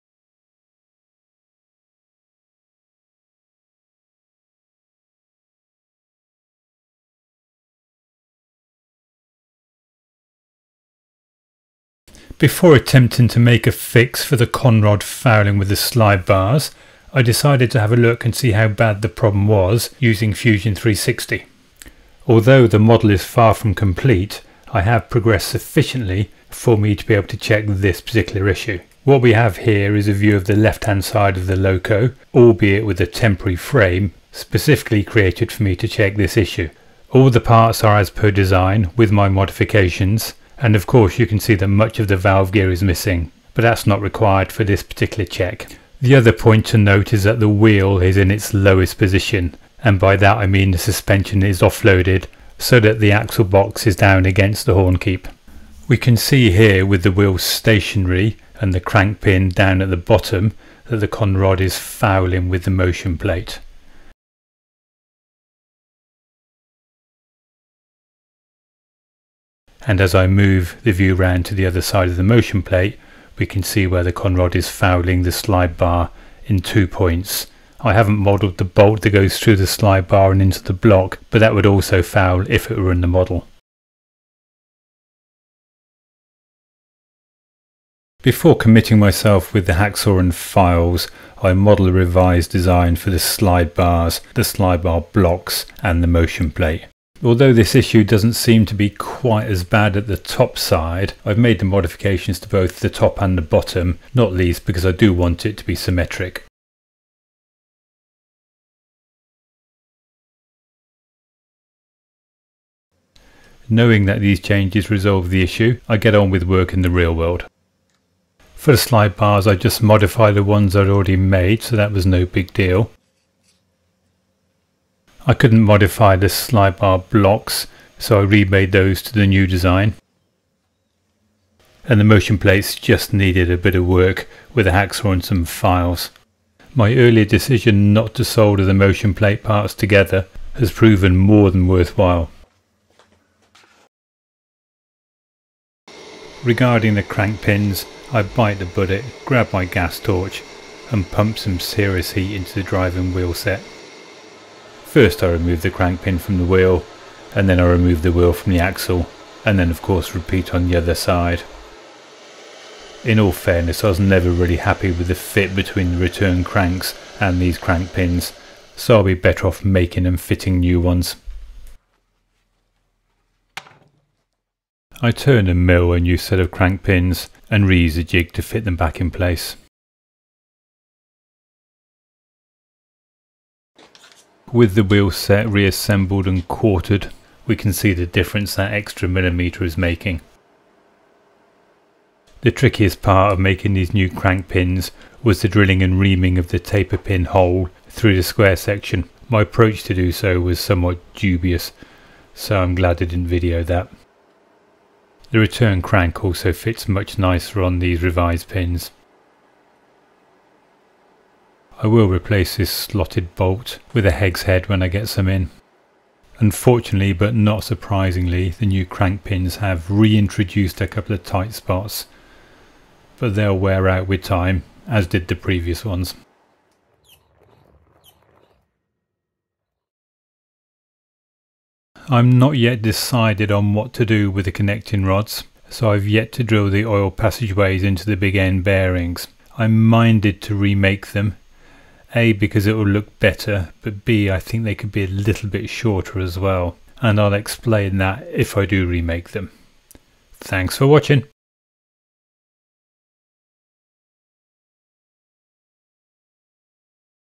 Part 39: Revised Slide Bars, new Driving Crankpins and further thinking on the Connecting Rods
Welcome to Part 39 of my long term series of videos covering the build of Don Young's Hunslet 0-6-0 Saddle Tank Locomotive in 5" Gauge.

In this short video I give an update on the issues encountered in Part 37.

Given the nature of these sorts of projects, new videos will be posted as I progress through the build but there will be no regular cadence, rather their frequency will be dependent upon time available and of course real progress on my part.